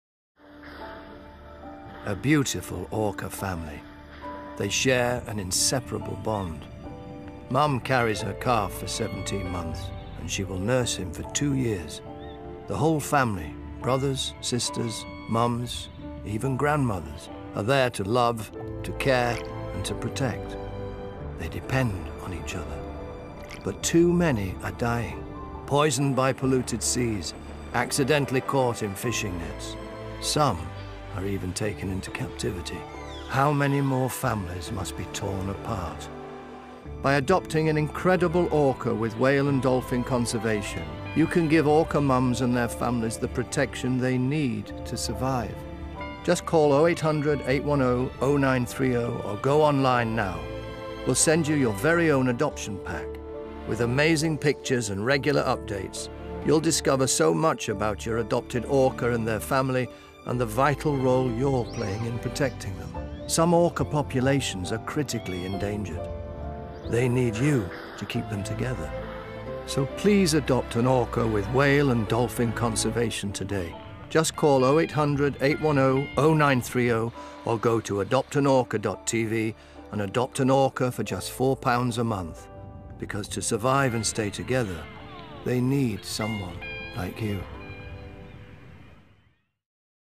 Adopt an orca from just £4 a month | Whale and Dolphin Conservation
Will you adopt an orca?

Orca families share an inseparable bond.  They depend on each other. But too many are dying. Poisoned by polluted seas. Accidentally caught in fishing nets. Some are even taken into captivity. Some orca populations are critically endangered. They need you to keep them together.

We’ll send you your very own adoption pack, with amazing pictures and a monthly e-newsletter you’ll discover so much about your adopted orca and their families and the vital role you’re playing in protecting them.